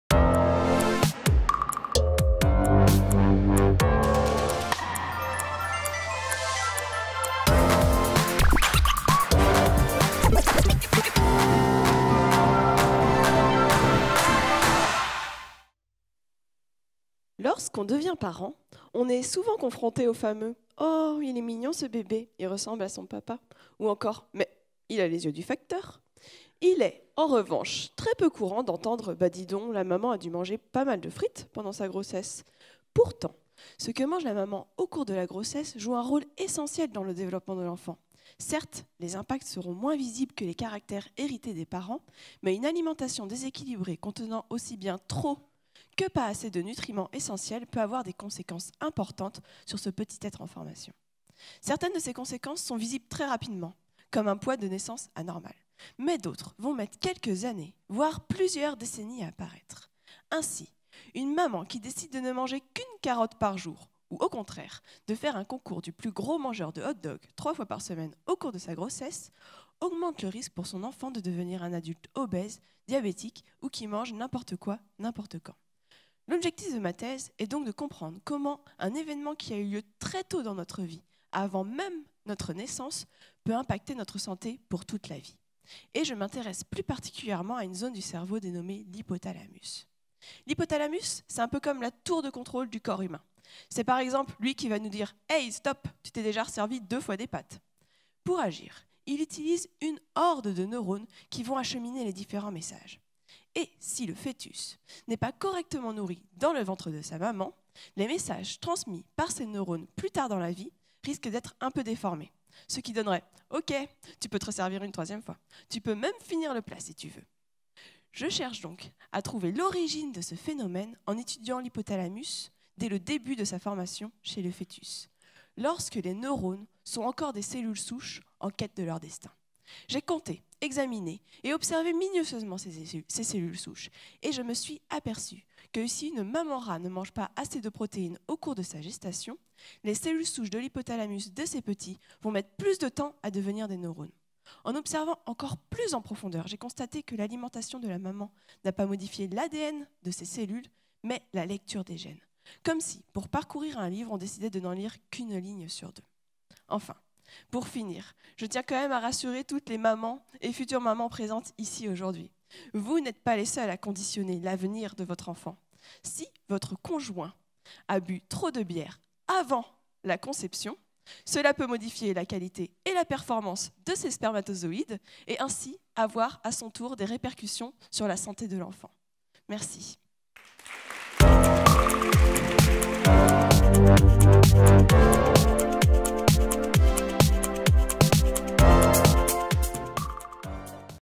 Morgane FRAPIN - MT180 Finale Bretagne / Pays de la Loire 2020
Son sujet de thèse : "impact d'une restriction protéique fœtale sur la différenciation neuronale chez le rat : rôle de l'épigénétique"

Son établissement : Université de Nantes